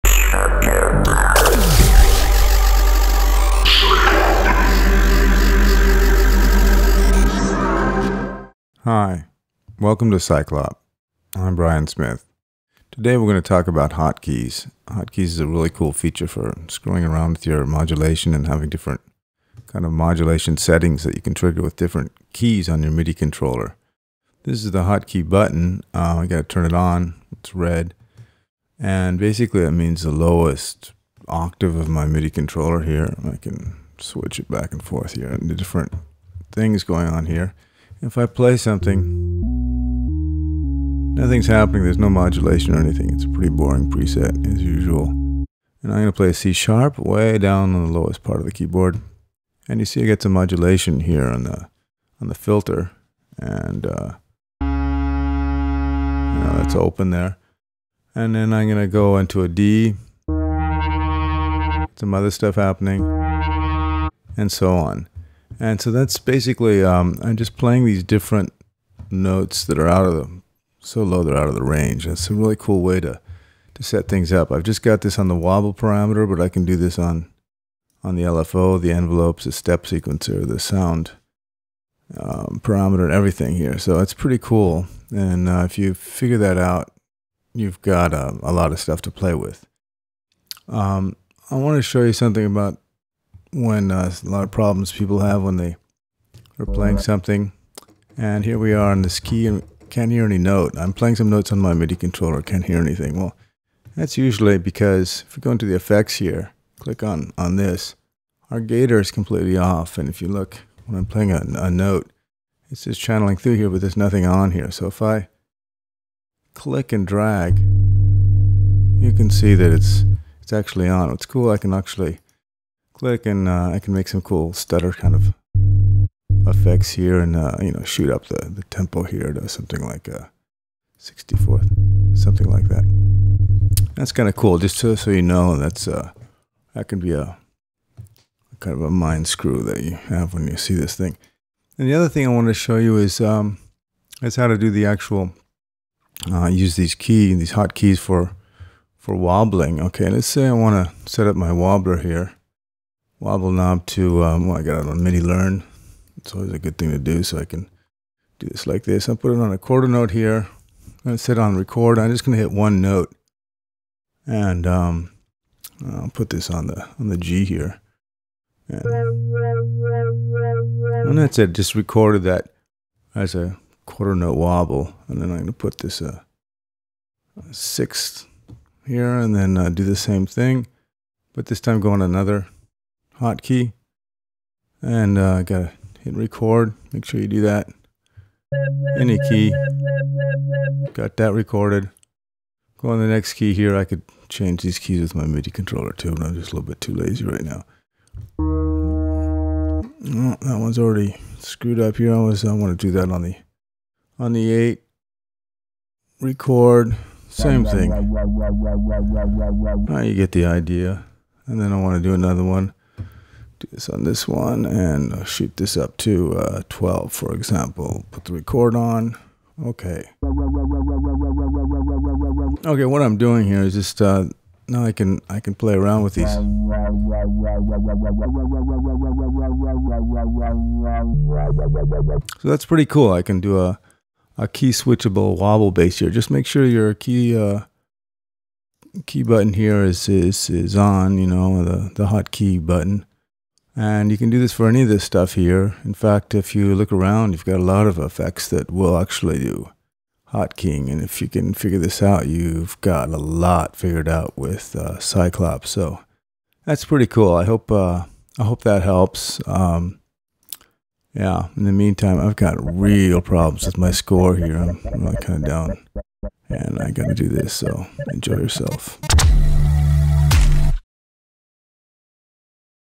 Cyclop Tutorial Nr. 3 "The HotKeys"
This Cyclop Synthesizer Tutorial will shed some light on how to get the best out of Cyclop using the HotKeys. The Hotkeys turn one sound to another by just pressing some special keys on your keyboard.

Cyclop is the new sound of bass.